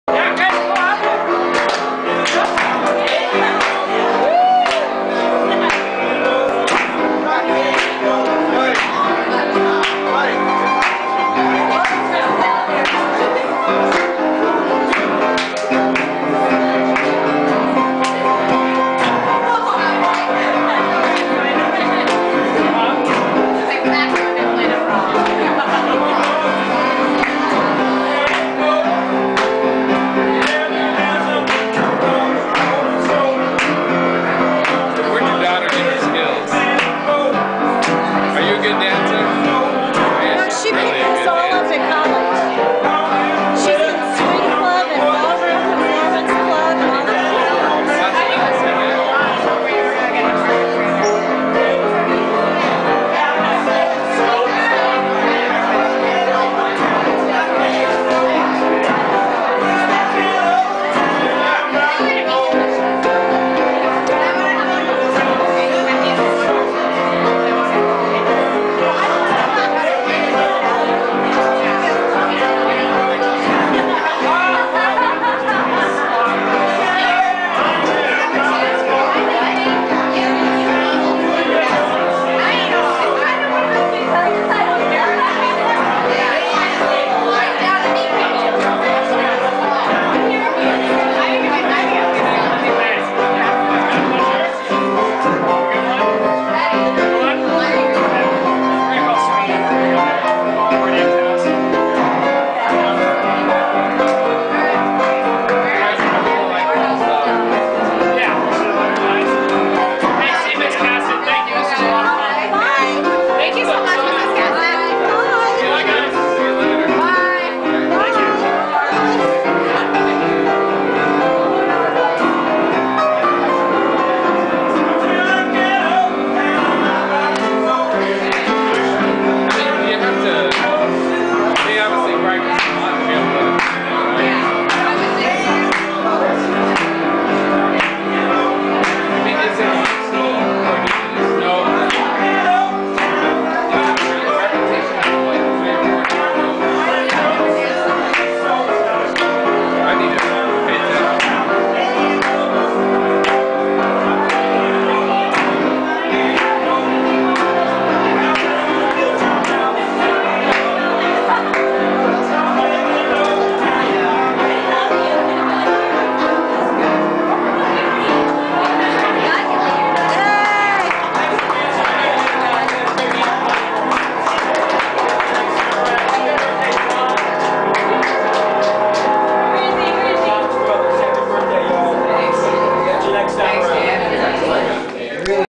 Dancing to Dan
It's not a quality video, I'll warn you-- the lighting is bad and there's people talking and we really didn't have much space to work with.  But Geoff wanted to see it, so...